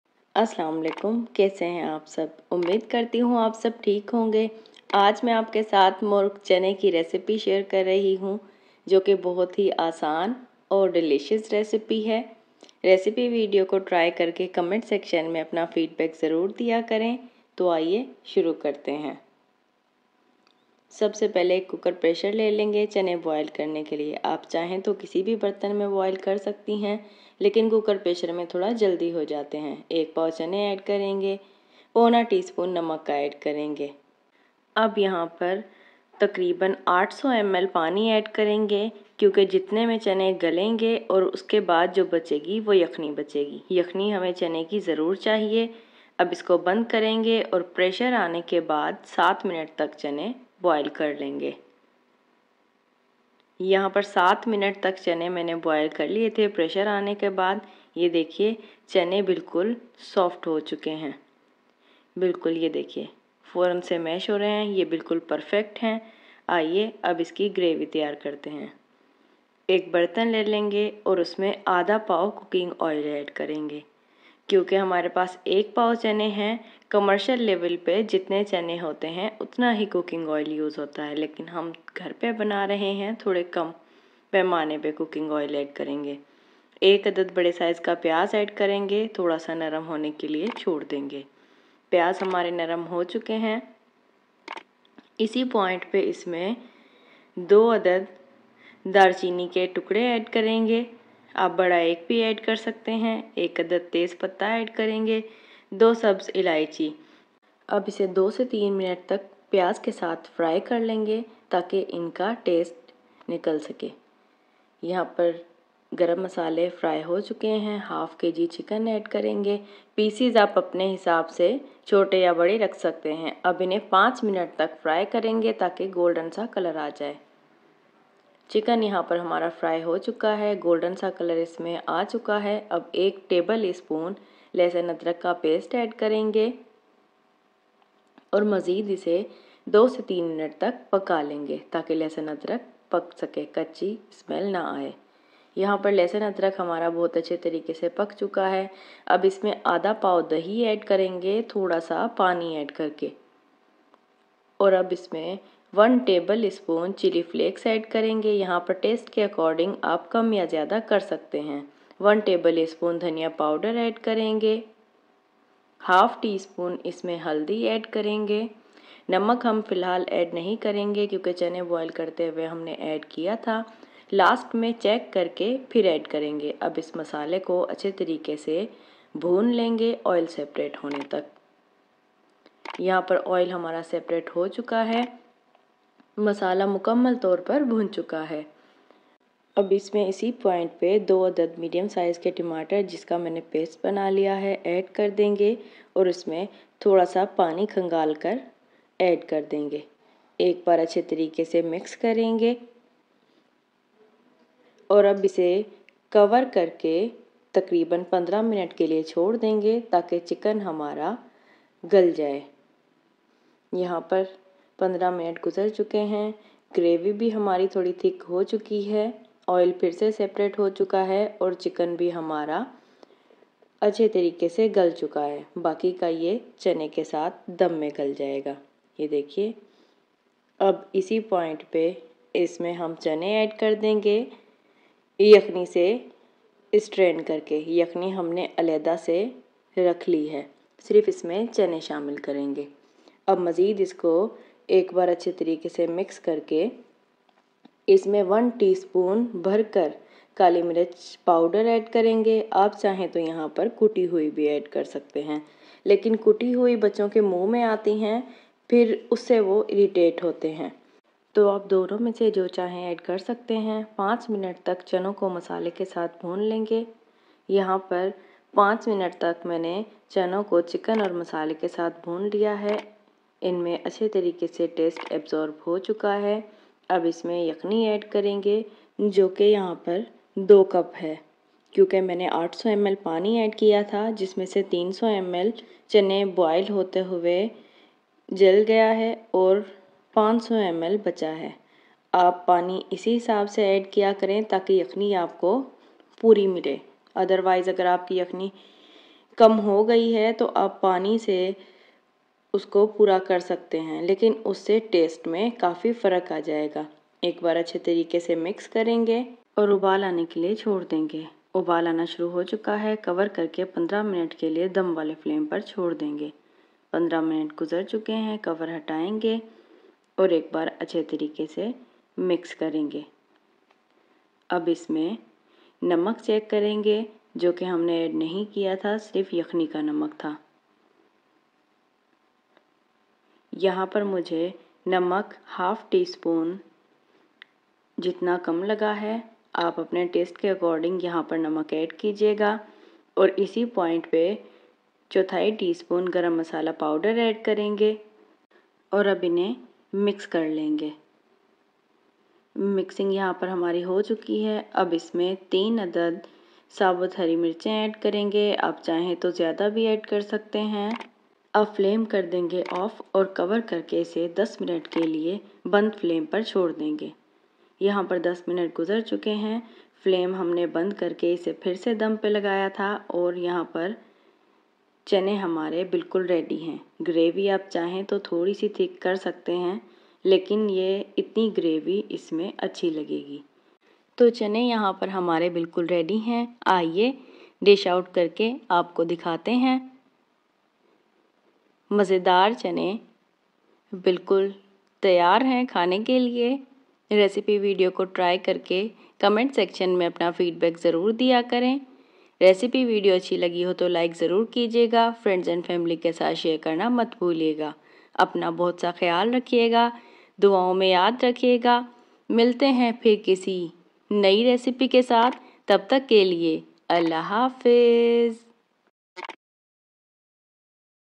Murgh Chane Recipe By Cook Like Maa | With English Subtitles | Chicken Chole | Lahori Murgh Chane
A tried and tested easy Murgh Chane recipe by Cook Like Maa. How can you go wrong with it.